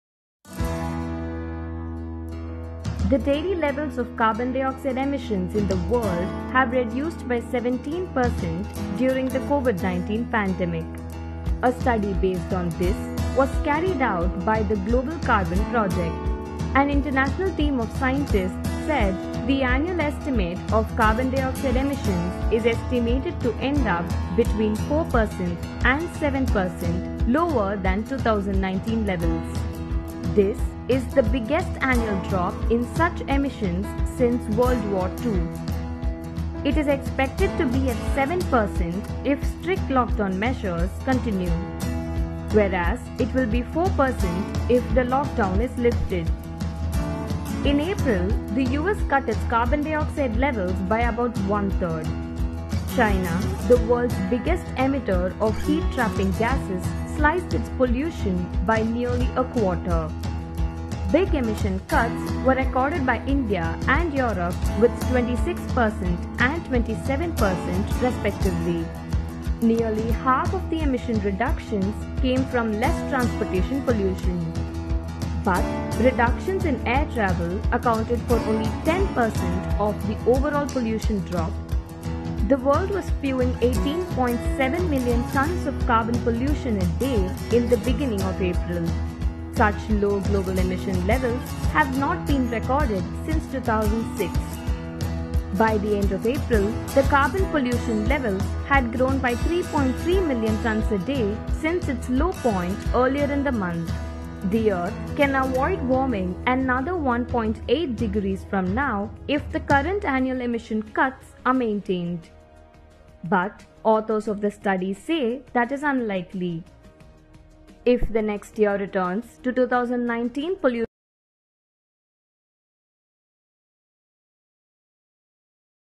World Carbon Pollution falls 17% during Pandemic Peak.
In their study of carbon dioxide emissions during the corona-virus pandemic, an international team of scientists calculated that pollution levels are heading back up — and for the year will end up between 4% and 7% lower than 2019 levels. That’s still the biggest annual drop in carbon emissions since World War II.